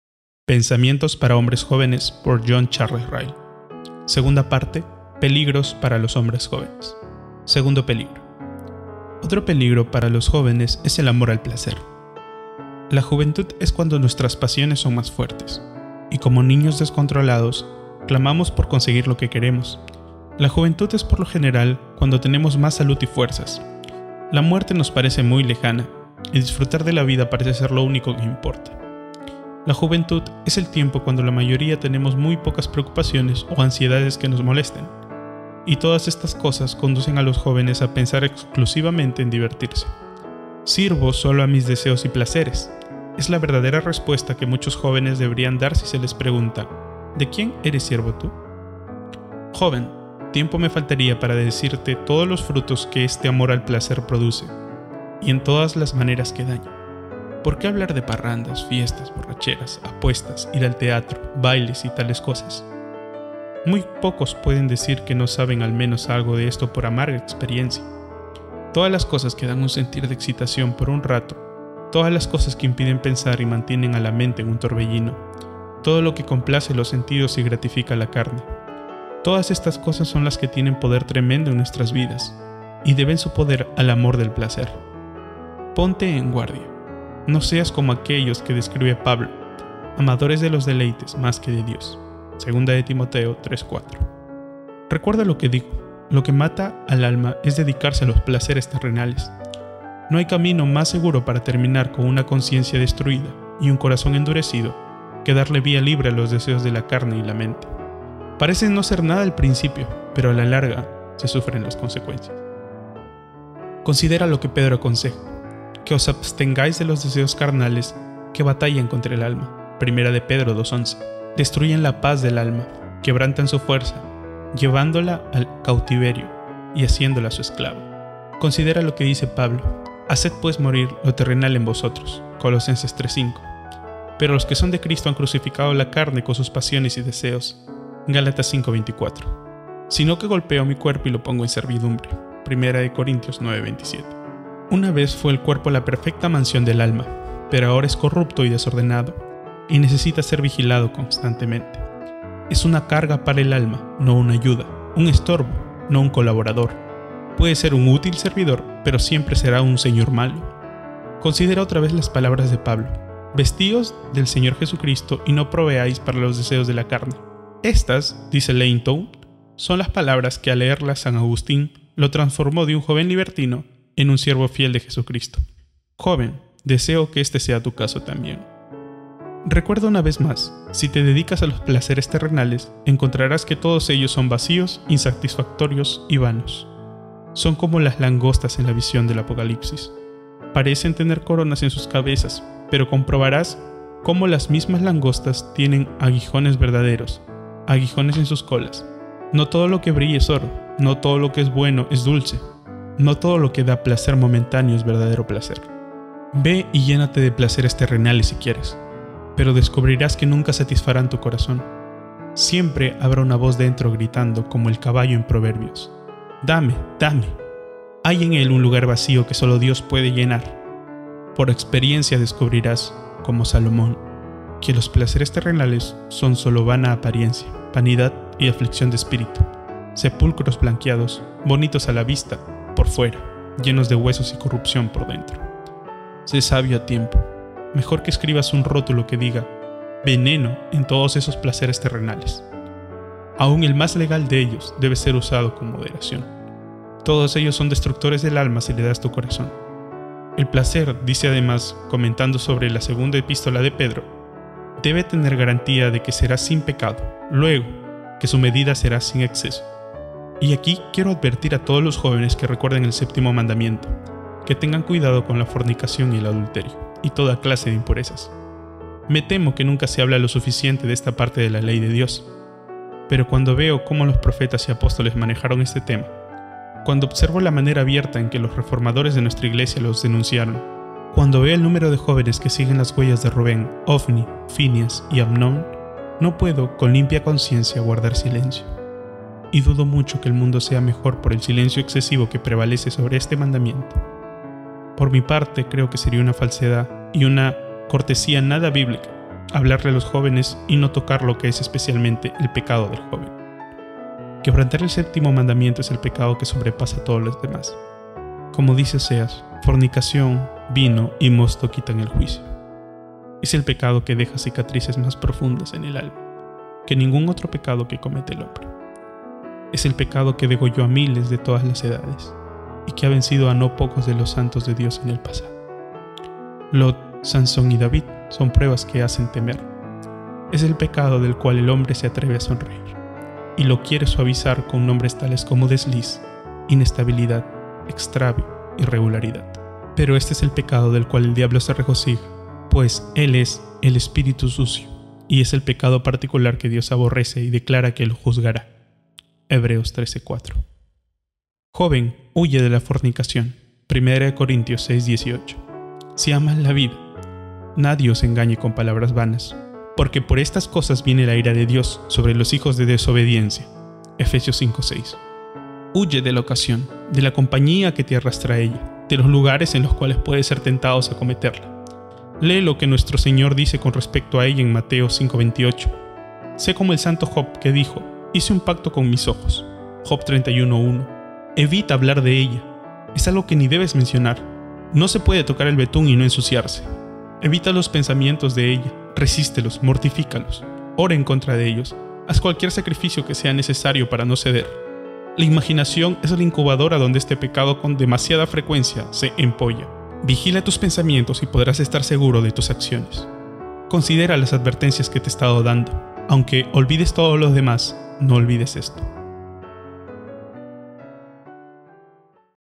AMOR AL PLACER - Peligros Juveniles | Pensamientos Para Jóvenes - J.C. Ryle (Audiolibro Cristiano)
Yo mismo me estoy haciendo viejo, pero hay unas cuantas cosas que recuerdo muy bien de mi juventud. Recuerdo vívidamente los gozos, temores, las tristezas, esperanzas, tentaciones y dificultades, las decisions equivocadas y los sentimientos mal fundados, los errores y las aspiraciones que rodean y acompañan la vida del joven. Si puedo decir algo para mantener a algún joven en el camino correcto y protegerlo de las faltas y los pecados, los cuales pueden dañar sus perspectivas en el tiempo y la eternidad, estaré muy agradecido.

Me propongo hacer cuatro cosas:
1. Mencionaré algunas razones generales por las cuales los jóvenes necesitan ser exhortados.
2. Haré notar algunos peligros especiales contra los cuales los jóvenes necesitan ser advertidos.
3. Daré algunos consejos generales que ruego a los jóvenes reciban.
4. Y estableceré algunas reglas de conducta especiales que aconsejo encarecidamente a los jóvenes seguir.

En cada uno de estos cuatro puntos, tengo algo que decir, y oro a Dios que lo que diga sea de bien para algún alma.

John Charles Ryle (10 de mayo de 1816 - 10 de junio de 1900) fue un obispo anglicano evangélico inglés. Fue el primer obispo anglicano de Liverpool .